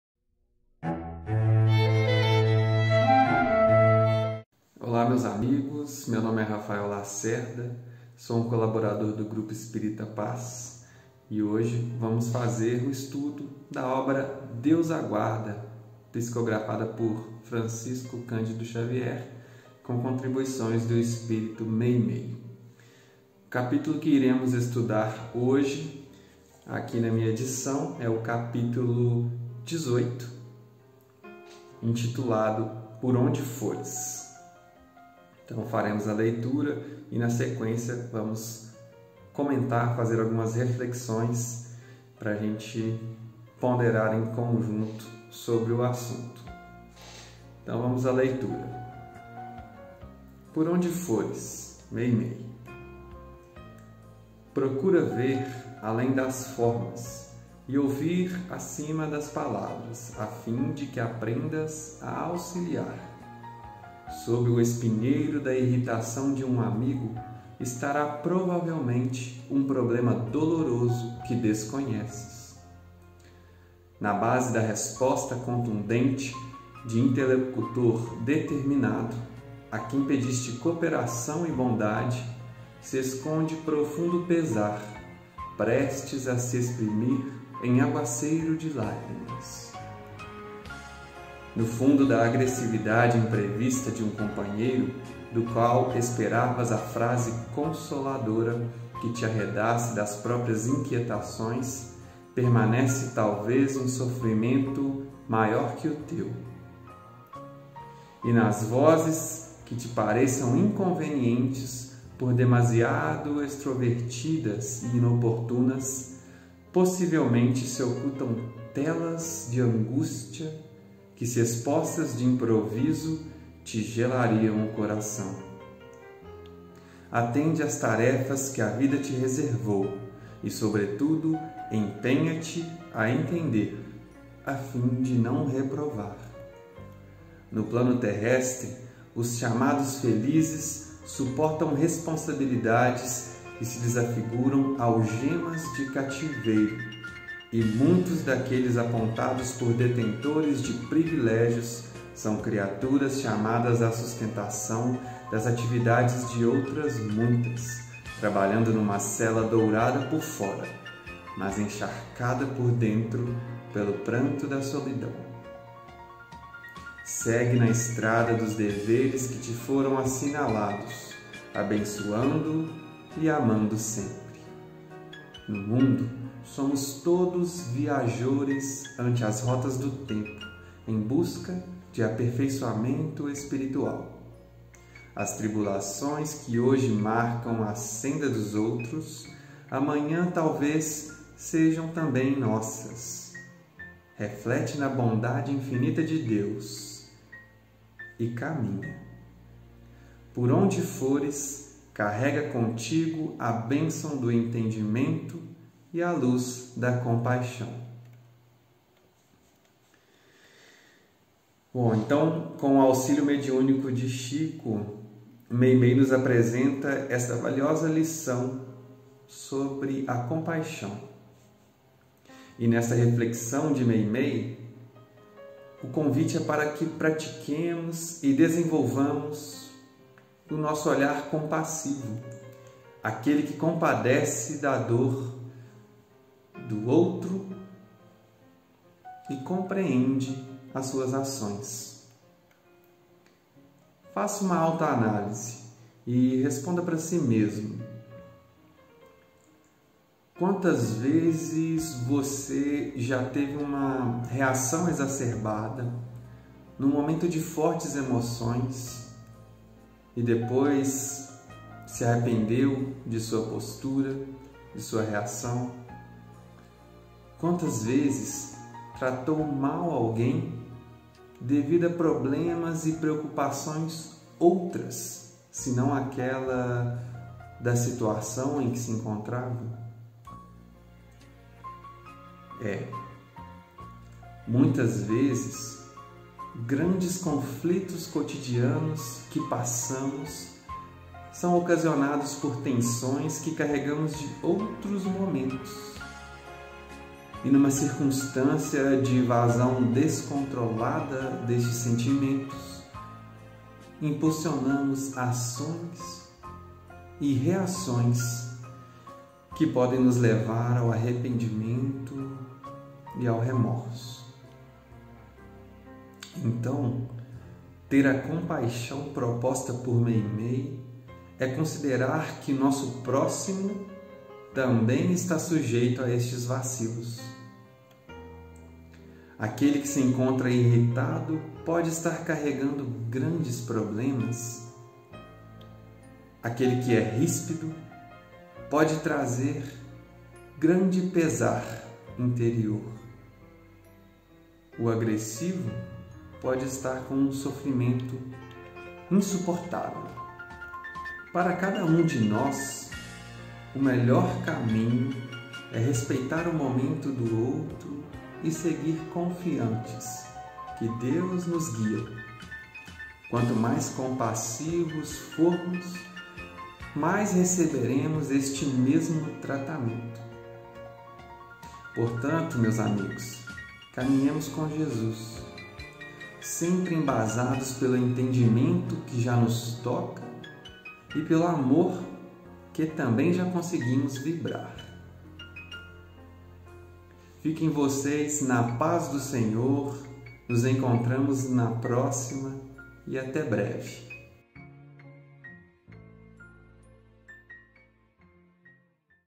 Deus Aguarda - 13 - Por Onde Fores
POR ONDE FORES

Procura ver, além das formas, e ouvir acima das palavras, a fim de que aprendas a auxiliar.
Sob o espinheiro da irritação de um amigo, estará provavelmente um problema doloroso que desconheces.
Na base da resposta contundente de interlocutor determinado, a quem pediste cooperação e bondade, se esconde profundo pesar, prestes a se exprimir em aguaceiro de lágrimas.
No fundo da agressividade imprevista de um companheiro, do qual esperavas a frase consoladora que te arredasse das próprias inquietações permanece talvez um sofrimento maior que o teu.
E nas vozes que te pareçam inconvenientes, por demasiado extrovertidas e inoportunas, possivelmente se ocultam telas de angústia que, se expostas de improviso, te gelariam o coração.
Atende as tarefas que a vida te reservou e, sobretudo, empenha-te a entender, a fim de não reprovar.
No plano Terrestre, os chamados felizes suportam responsabilidades que se lhes afiguram algemas de cativeiro e muitos daqueles apontados por detentores de privilégios são criaturas chamadas à sustentação das atividades de outras muitas, trabalhando numa cela dourada por fora, mas encharcada por dentro pelo pranto da solidão.
Segue na estrada dos deveres que te foram assinalados, abençoando e amando sempre.
No mundo, somos todos viajores ante as rotas do tempo, em busca de aperfeiçoamento espiritual.
As tribulações que hoje marcam a senda dos outros, amanhã talvez sejam também nossas.
Reflete na Bondade Infinita de Deus e caminha.
Por onde fores, carrega contigo a bênção do entendimento e a luz da compaixão.

Meimei